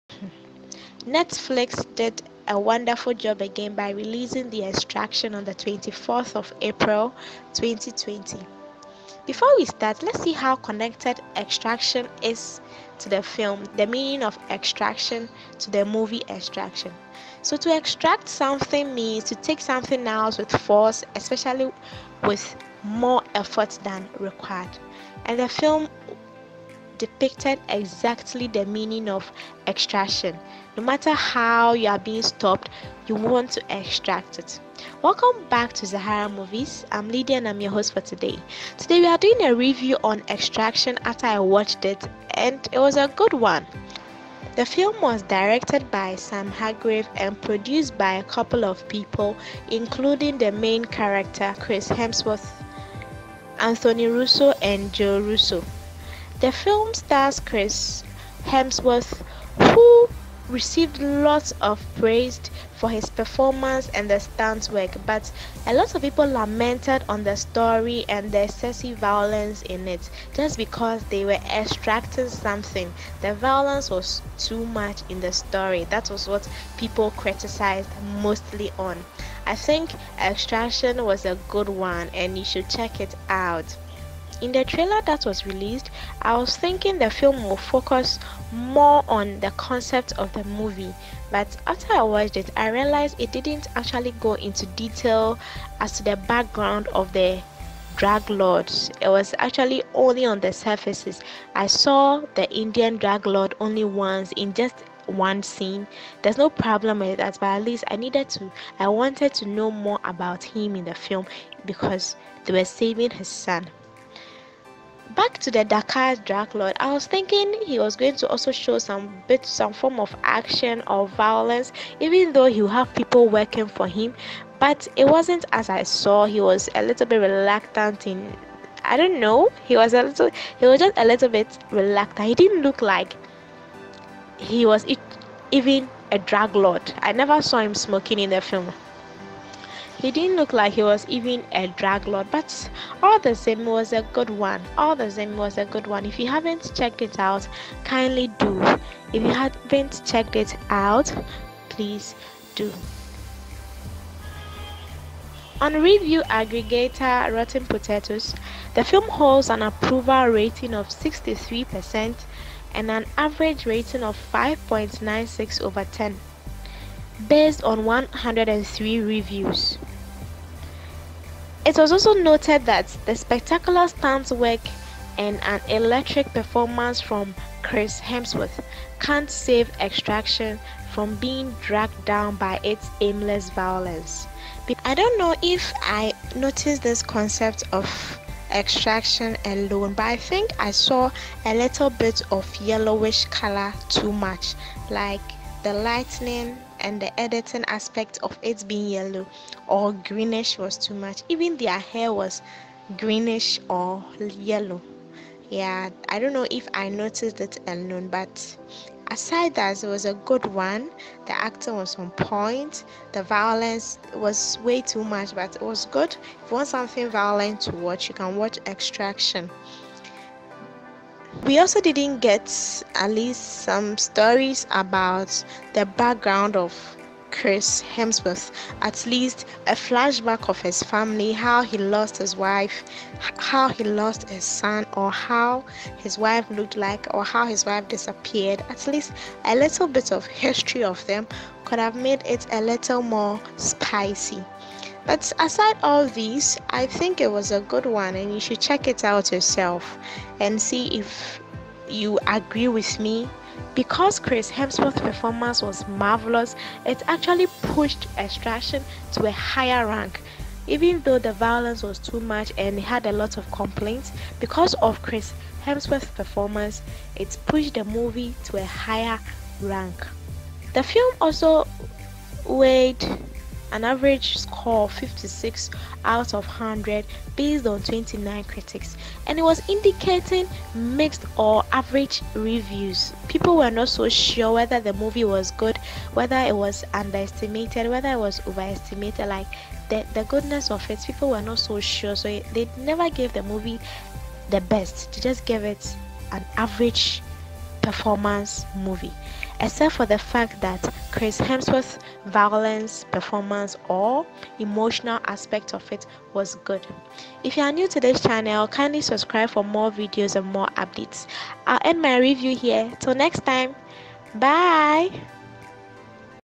Extraction (2020)- Netflix Movie Review
Hey Guys, Chris Hemsworth did a marvelous performance in Extraction.